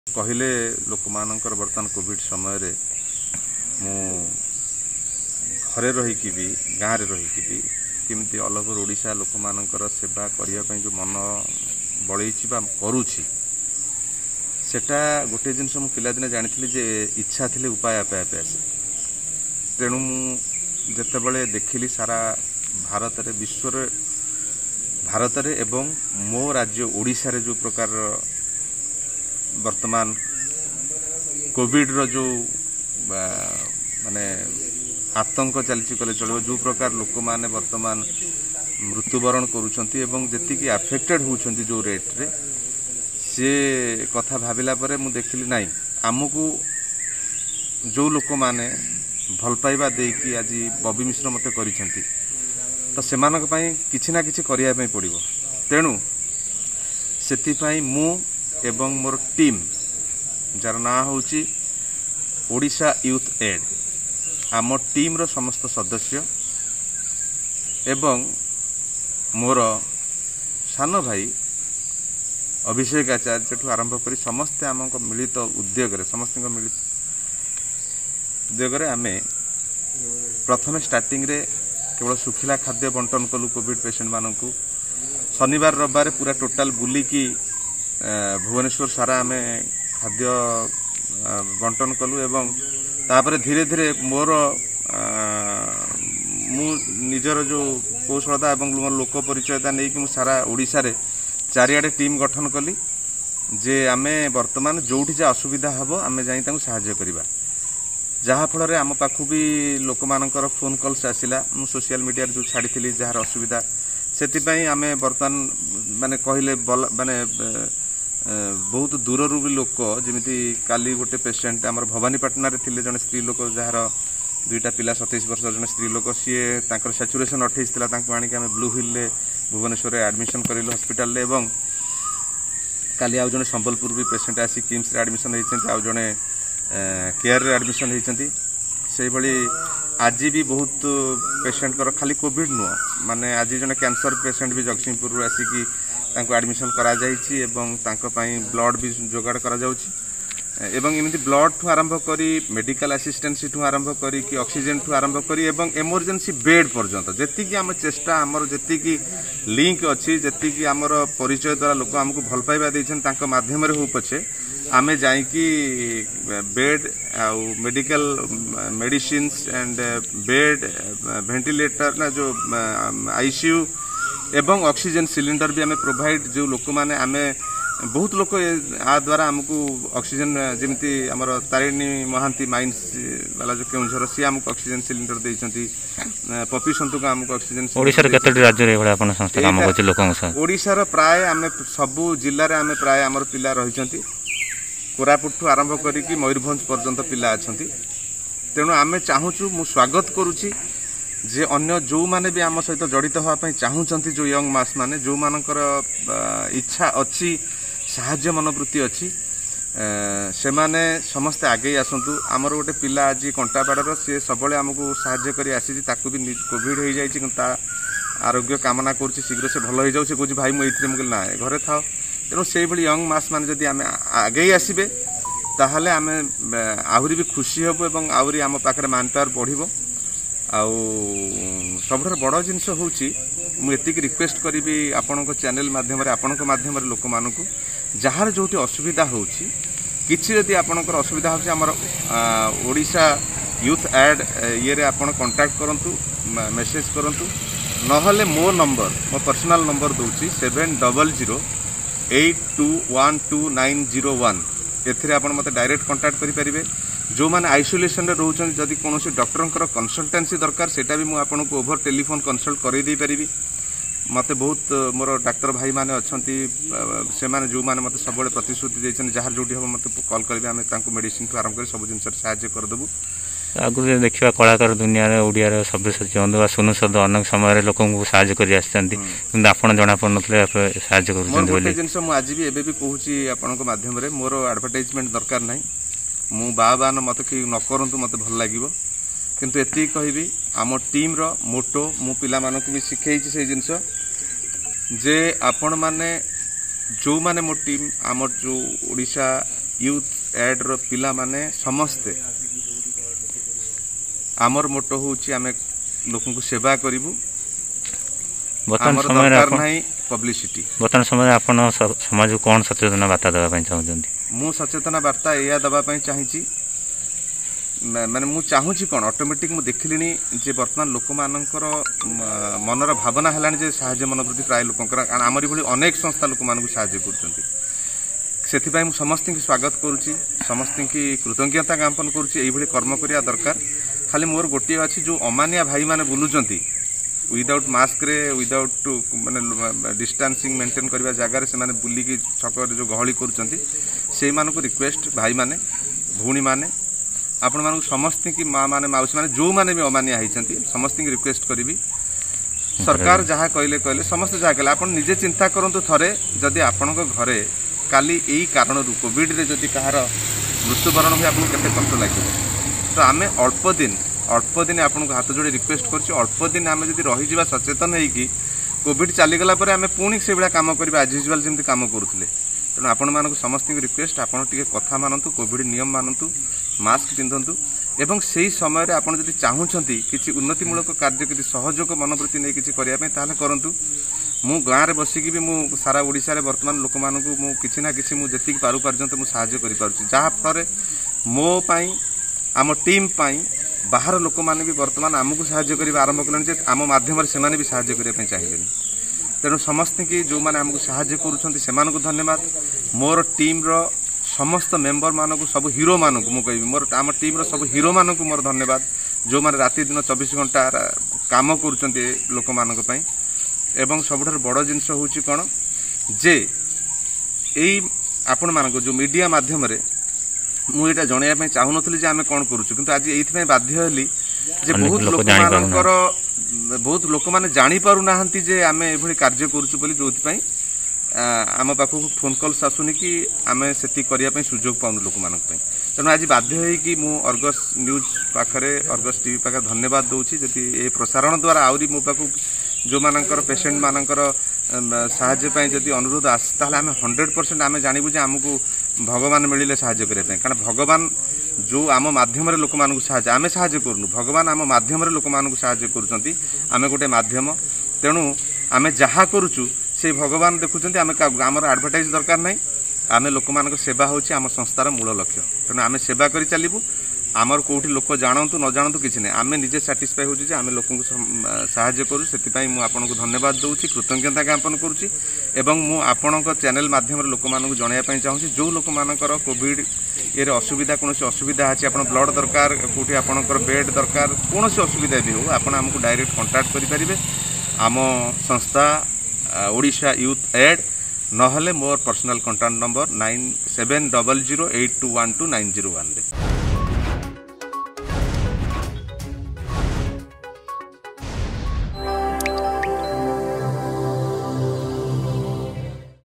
ଇଚ୍ଛା ଥିଲେ ଉପାୟ ଆପେ ଆପେ ଆସେ: ଅଭିନେତା ବବି ମିଶ୍ର।
ବବି ମିଶ୍ରଙ୍କ ଓଡିଶା ୟୁଥ୍ ଏଡ୍ ଟିମ୍ କରୋନା ରୋଗୀଙ୍କୁ ବାଣ୍ଟୁଛନ୍ତି ଖାଦ୍ୟ।